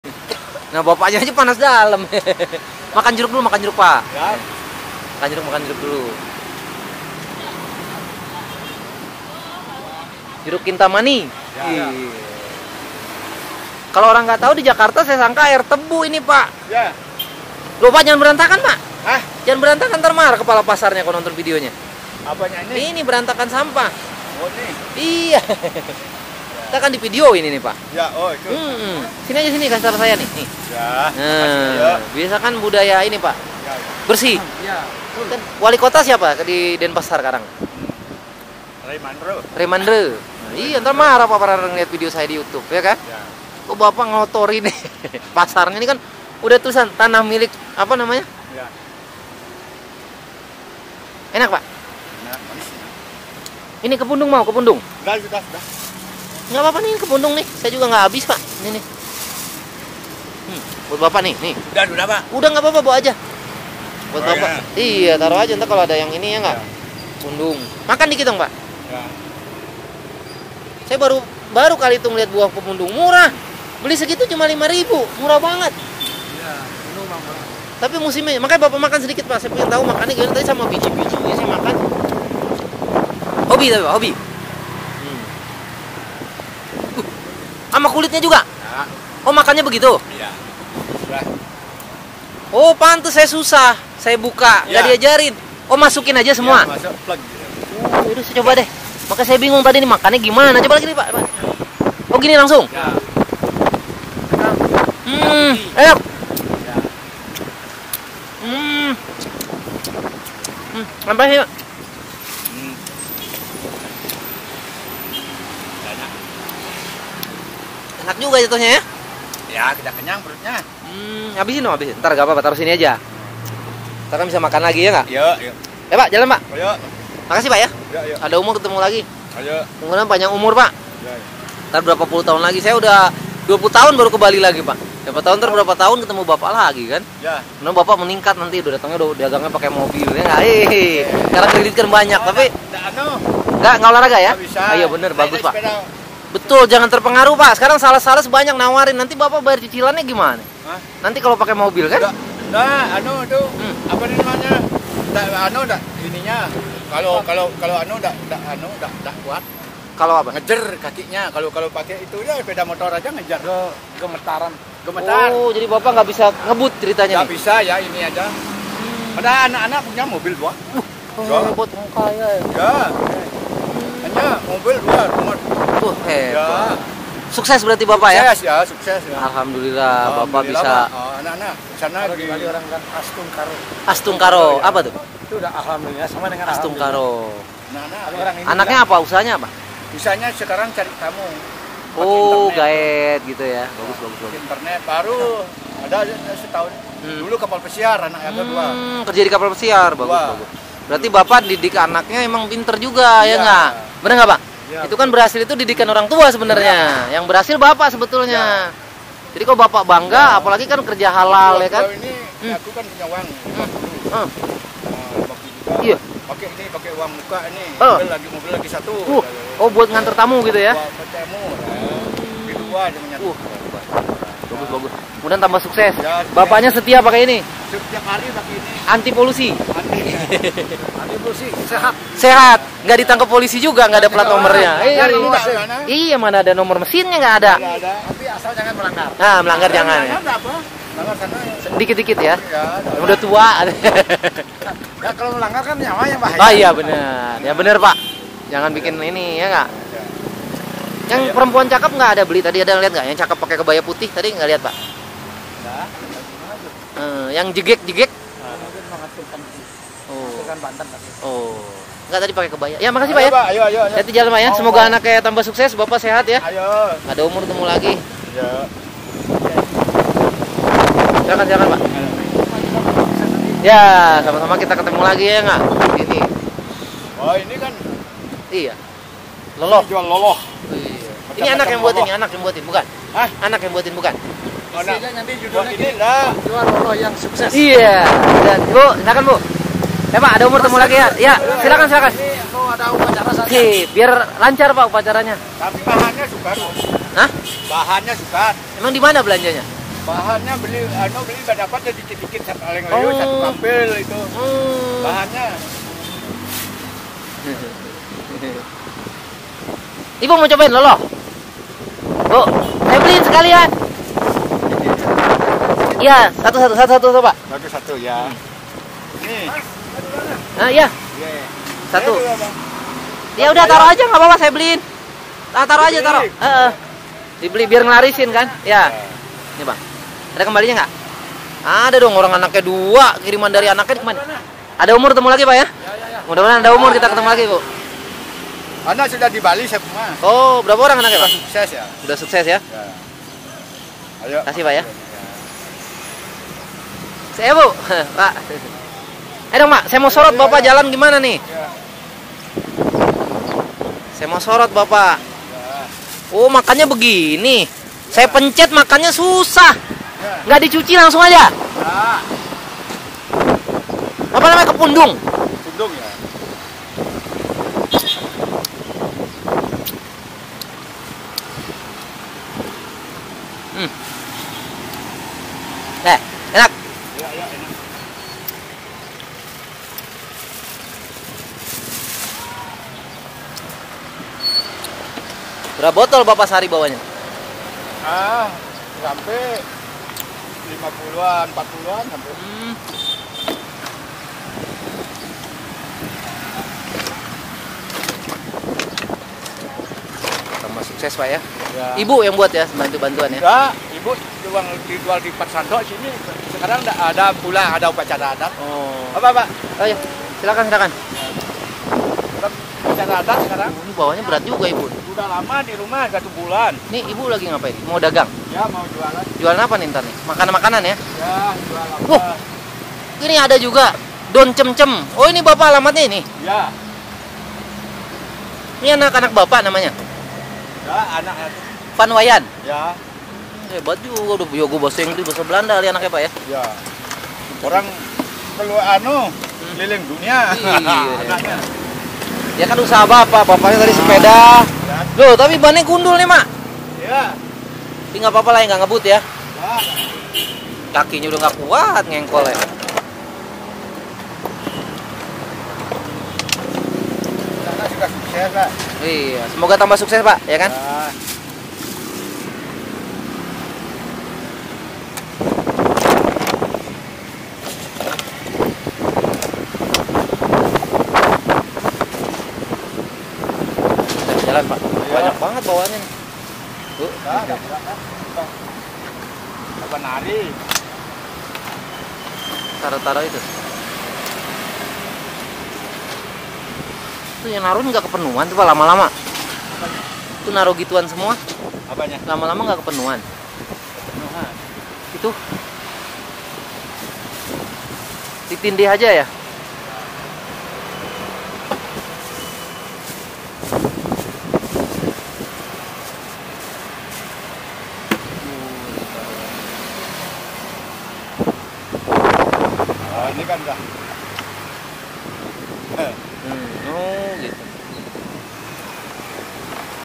Indonesia Bali Street Food 1810 Part.2 Loloh Cemcem Kayu Manis Rujak Tibah YN010845
We can buy Loloh Cemcem Kayu Manis Rujak Tibah only Rp.5.000 (38 Cents USD) per bottle the taste is so good.
hi TTM:
Rasa Loloh Cemcem Kayu Manis Rujak Tibah memang enak banget, silahkan datang aja langsung ke Pasar Badung Penampungan di Bekas Tiara Grosir Jalan Cokroaminoto seberang Hotel Pop Harris Denpasar Bali, thanks for watching:).